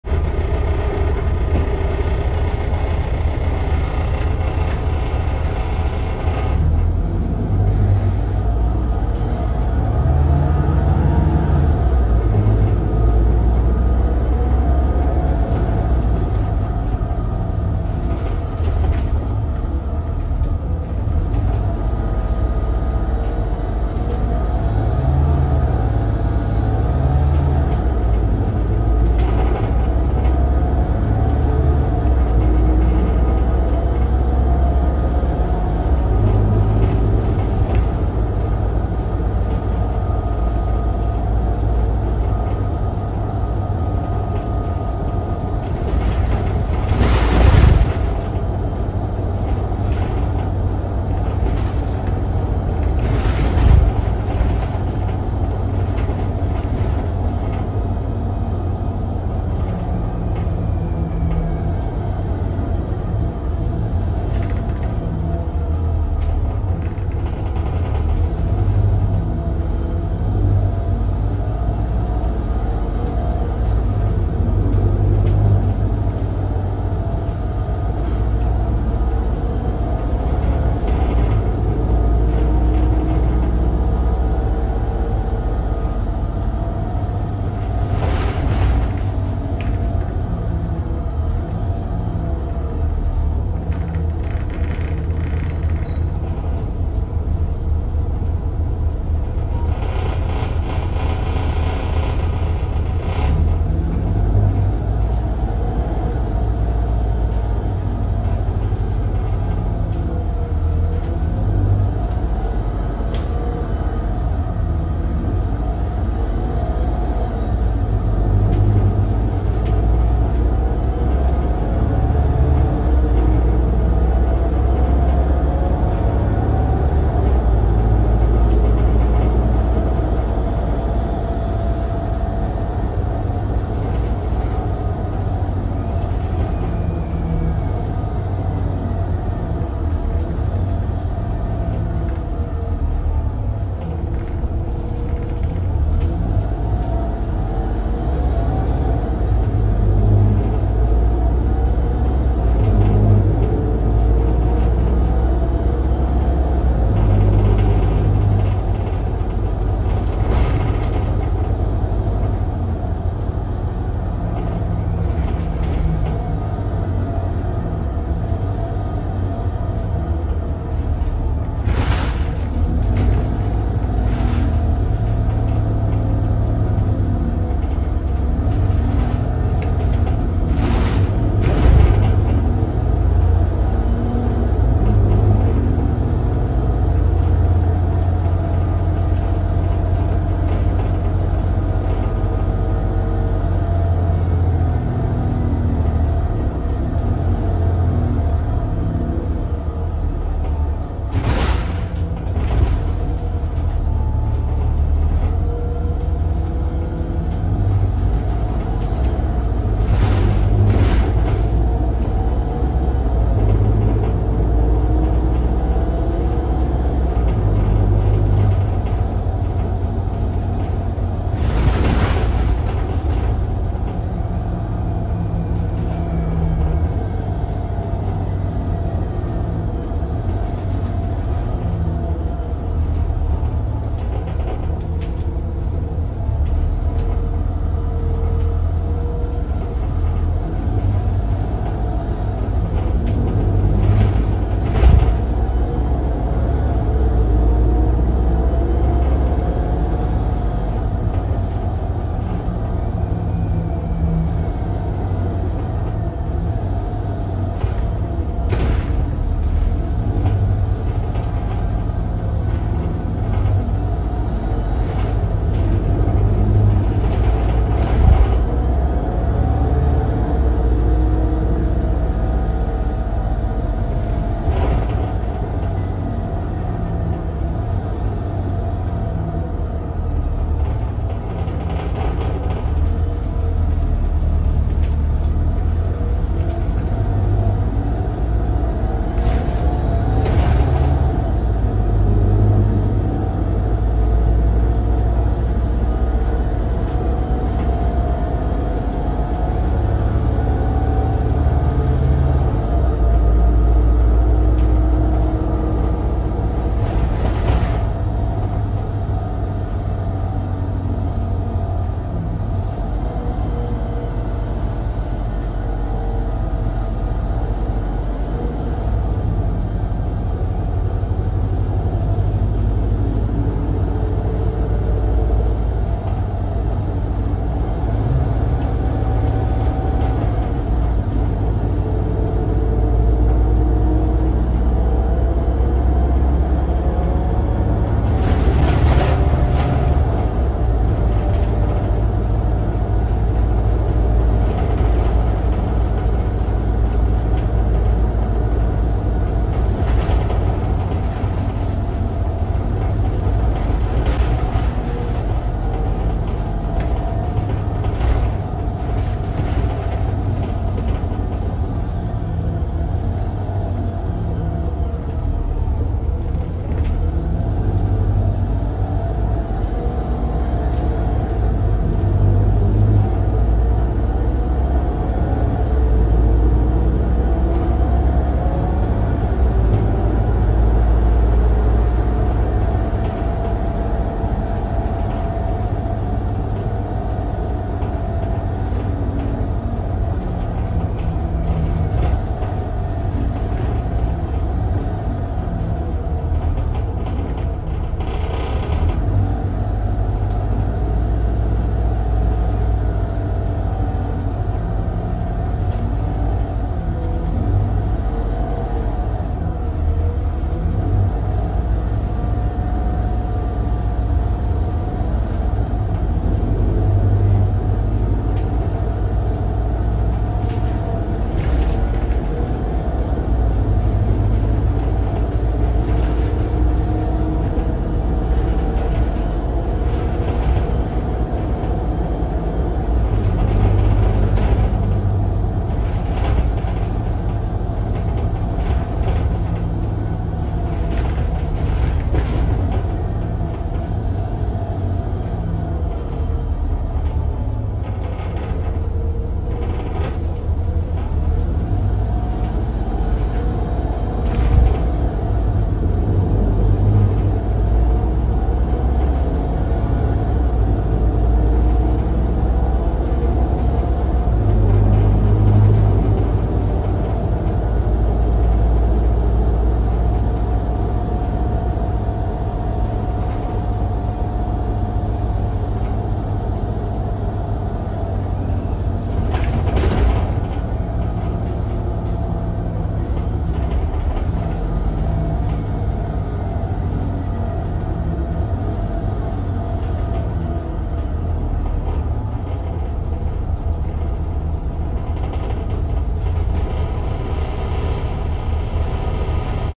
Transperth TP2173 - Volvo B7RLE (Voith)
Transperth Bus No. 2173
Operator: Swan Transit
Chassis: Volvo B7RLE
Bodywork: Volgren "Optimus"
Entered service: 2014
Engine: Volvo D7E290 6cyl 7.145L 213kW/290PS turbocharged & intercooled SCR diesel
Gearbox: Voith D864.5 4sp automatic
Status: In service, operating in the Joondalup Contract Area
Route: 374 - Mirrabooka Bus Stn to Whitfords Stn